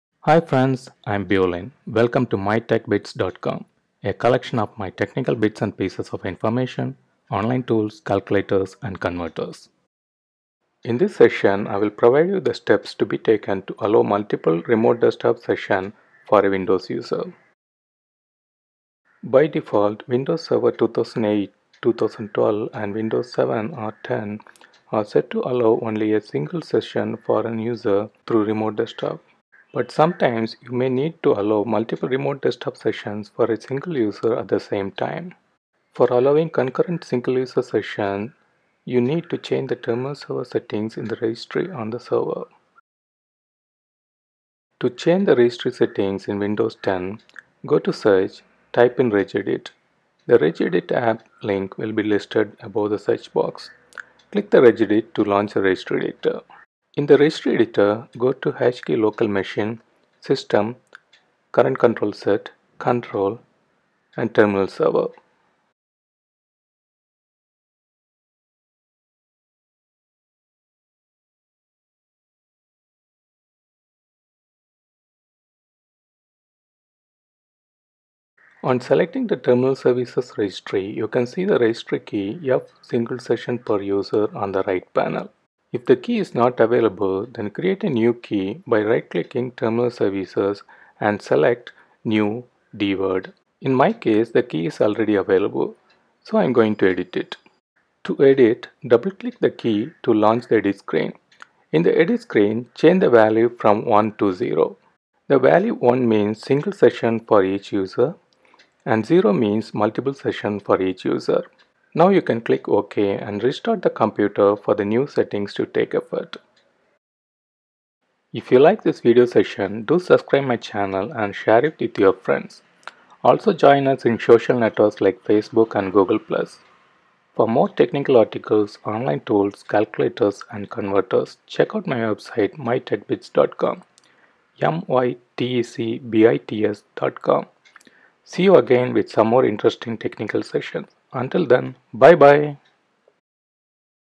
Multiple RDP Sessions For A Windows User | Windows #1
How to allow multiple remote desktop sessions (RDP Sessions Or TS Sessions) for the same user at the same time on a Windows Server 2008, 2012 and Windows 7 or 10.